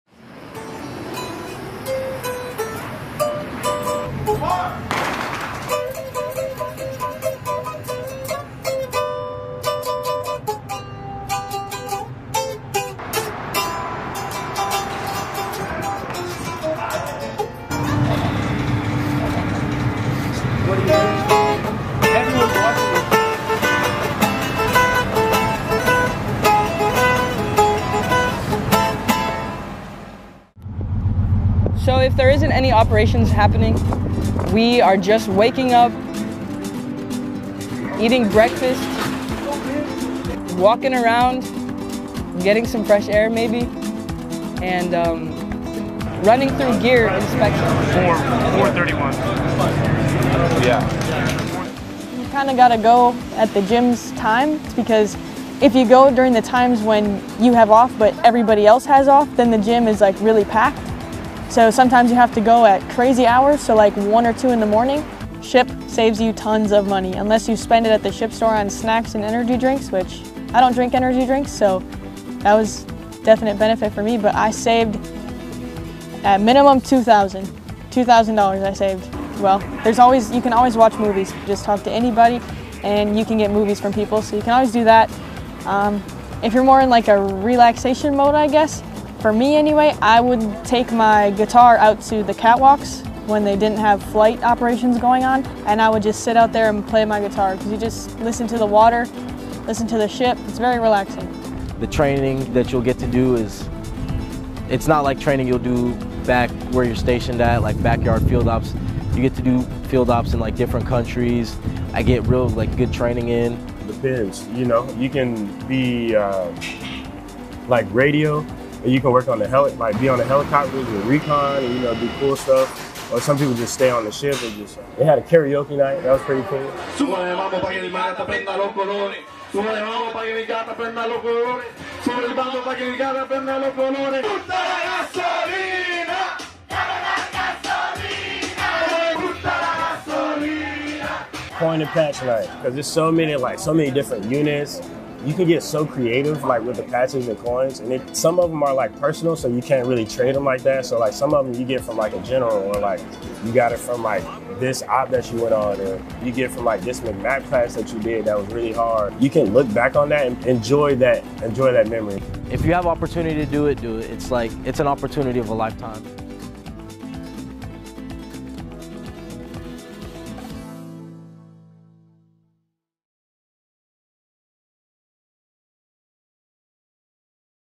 U.S. Marines & Navy Sailors LIVE on Ships in the Philippine Sea
U.S. Marines and U.S. Navy ships stationed in the Philippine Sea. Experience firsthand the daily life and duties of these brave servicemen and women as they share their stories while underway as part of the America Amphibious Ready Group. Get a unique insight into their experiences, missions, and camaraderie in this exclusive behind-the-scenes look at life on the seas. Don't miss out on this extraordinary opportunity to witness the dedication and sacrifice of our troops firsthand.

U.S. Marines and U.S. Navy Sailors embarked on ships part of the America Amphibious Ready Group, share their experiences while underway in the Philippine Sea, Jan. 29 - March 17, 2024.

31st MEU Life on Ship
PHILIPPINE SEA, JAPAN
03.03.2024

 This video production serves to display the everyday routines of servicemembers on ship while at sea. The 31st MEU is operating aboard ships of the USS America Amphibious Ready Group in the 7th Fleet area of operations, the U.S. Navy’s largest forward-deployed numbered fleet, and routinely interacts and operates with allies and partners in preserving a free and open Indo-Pacific region. (U.S. Marine Corps photo by Cpl. Juan K. Maldonado)

Film Credits: Video by Cpl. Juan Maldonado
31st Marine Expeditionary Unit